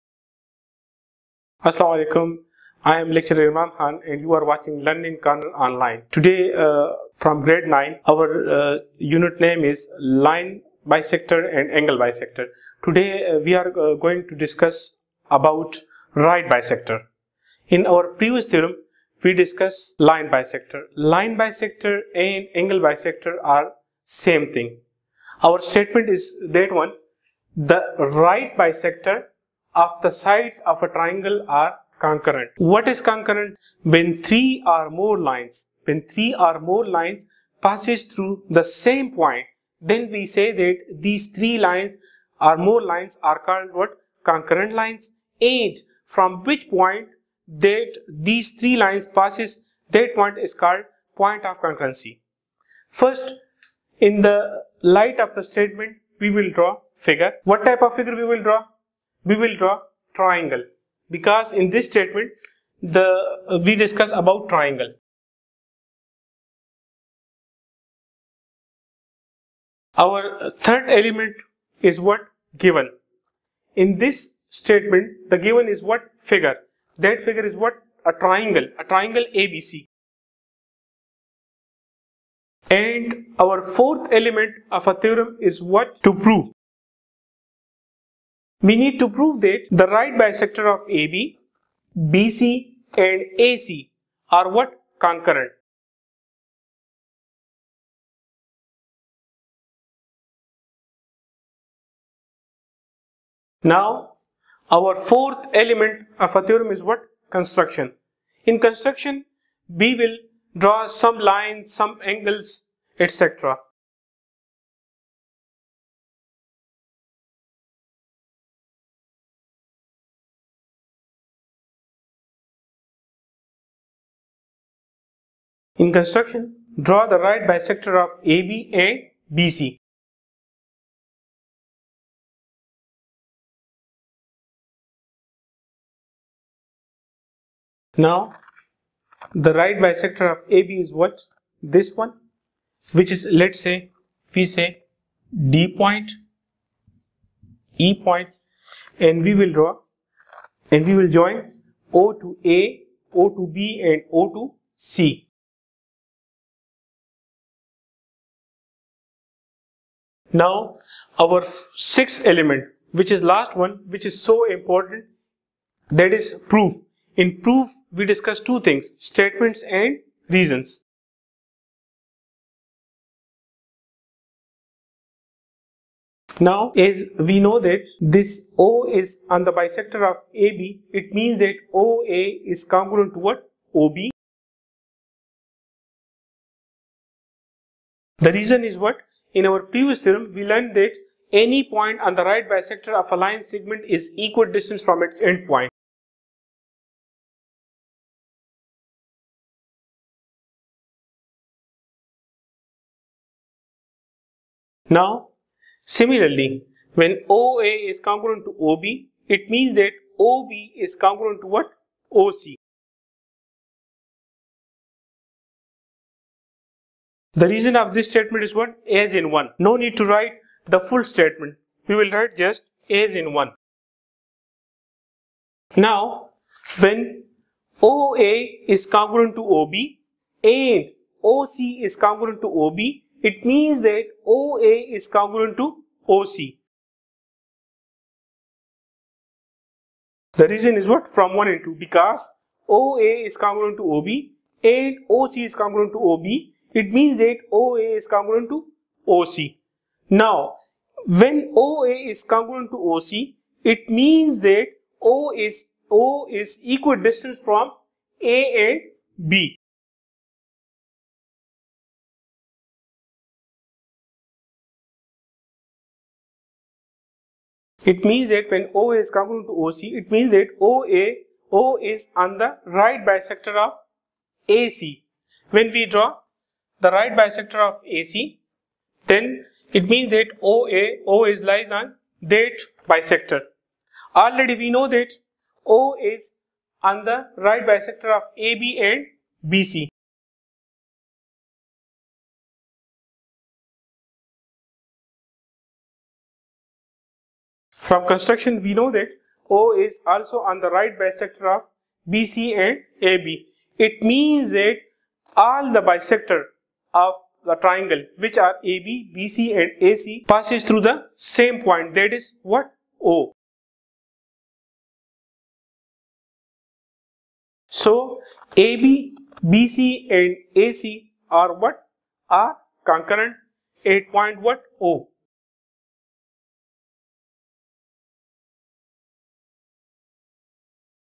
The right bisector of the sides of a triangle are concurrent
Class 9
Subject: Maths
Chapter # 12: Line bisector and Angle bisector
Theorem: 12.1.3